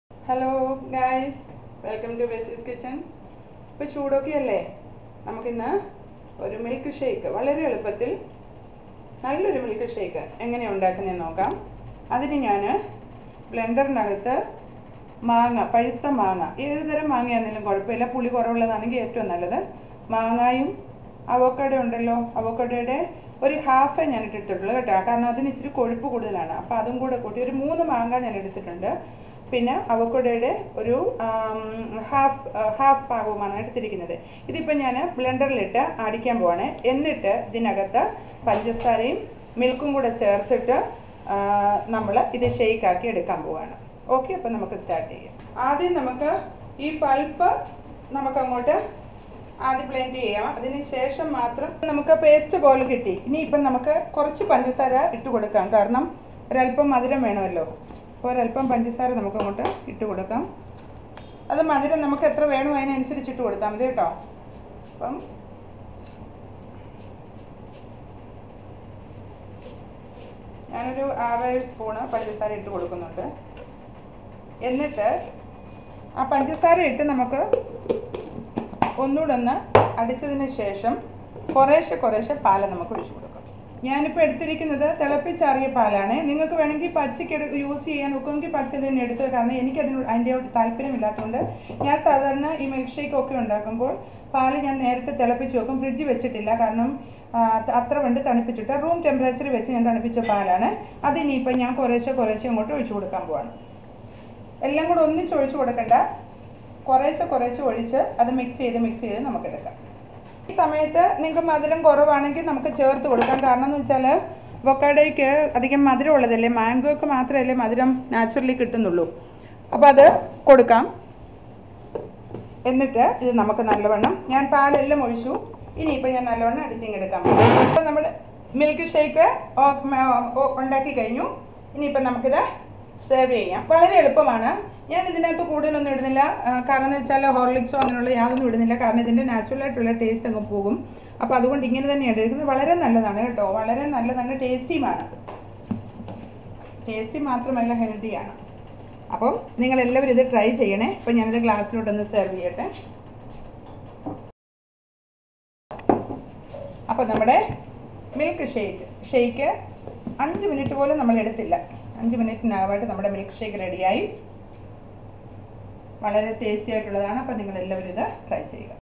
How to make Special Milkshake with Mango & Avocado # Ep 47
How to make Special Milkshake with Mango & Avocado
This video is about how to make Special Milkshake with Mango & Avocado pure homemade and best for summer.